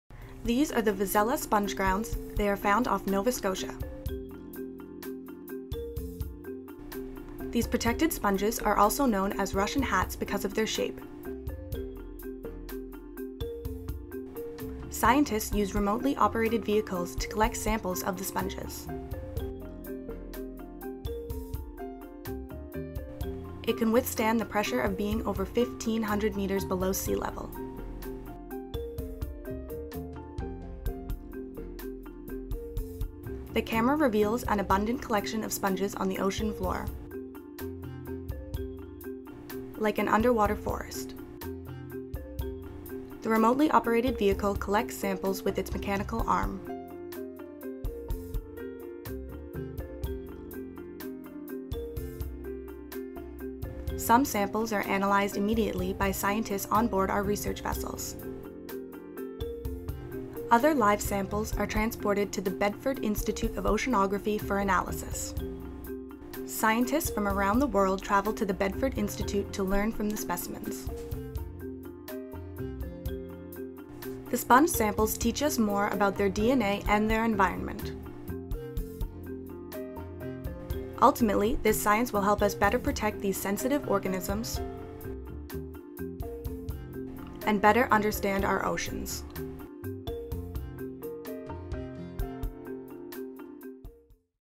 Deep-Sea Vazella Sponges
Found off the coast of Nova Scotia, the Emerald Basin is home to the unique Vazella Sponges. Commonly known as “Russian Hats” because of their odd yet familiar appearance, these sponges were examined as part of a five-week deep sea exploration mission of the Atlantic Ocean.  Watch as Canadian and international scientists collect live samples of the Vazella Sponge, which will be analysed to foster the study of deep-sea sponges and their ecosystem.